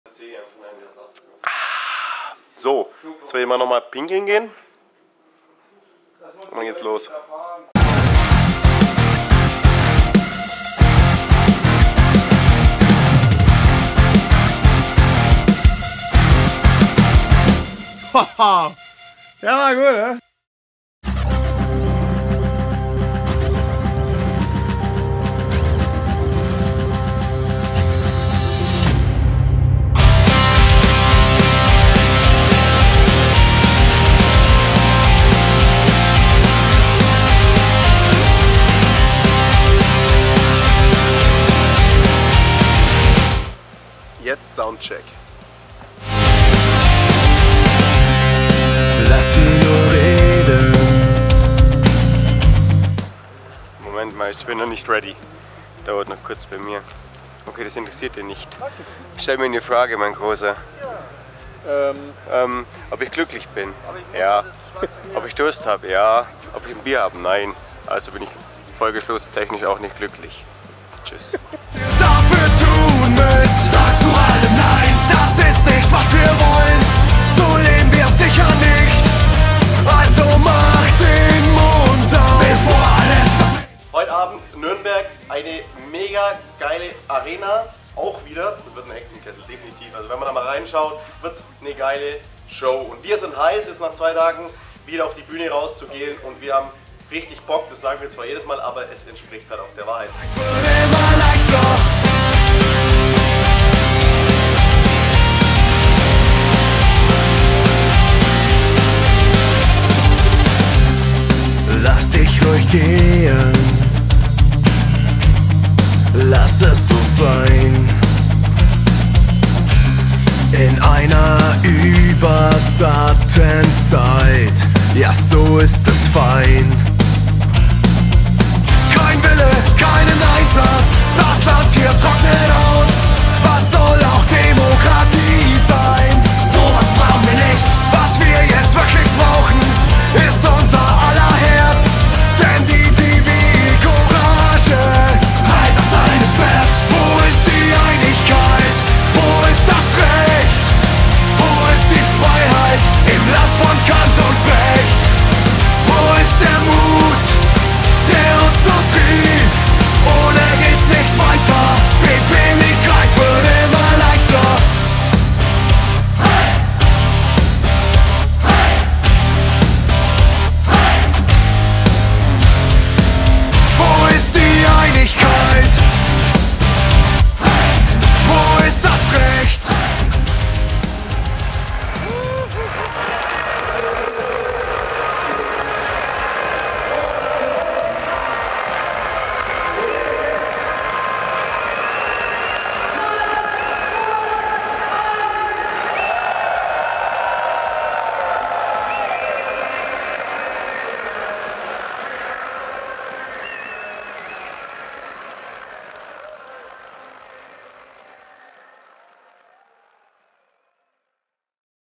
Grenzenlos - Tour Vlog Nürnberg, Arena Nürnberger Versicherungen 25.05.23
Heute gibt es den nächsten VLOG für euch!💜
5: VLOG: DAS WAR NÜRNBERG🔥 Supporttour Frei.Wild "Wir schaffen Deutsch.Land"

Sichert Euch eine der letzten verfügbaren und streng limitierten, nummerierten Fanboxen!

Eure Band Grenzenlos